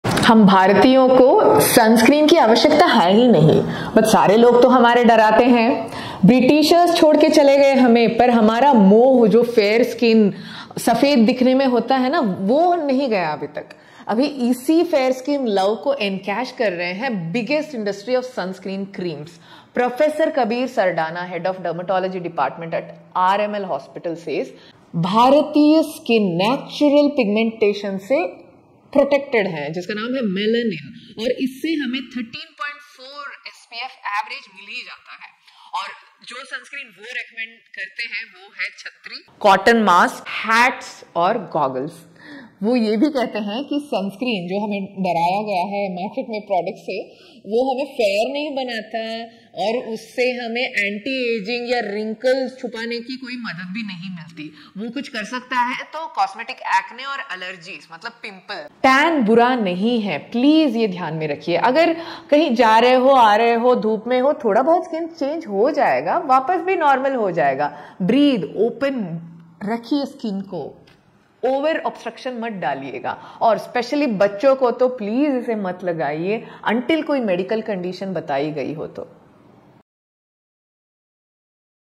भारतीयों को सनस्क्रीन की ज़रूरत है? Do Indians need sunscreen?
☀️क्या भारतीयों को सनस्क्रीन की ज़रूरत है?

🌤बिलकुल नहीं, क्योंकि हम भारतीयों में पहले से ही मेलेनिन (melanin) होती है जो हमें सूरज की हानिकारक किरणों से बचाती है

🌟इस वीडियों में अधिक जानकारी प्राप्त करें

💥अपने Tan को अपनाएं और शान से रहे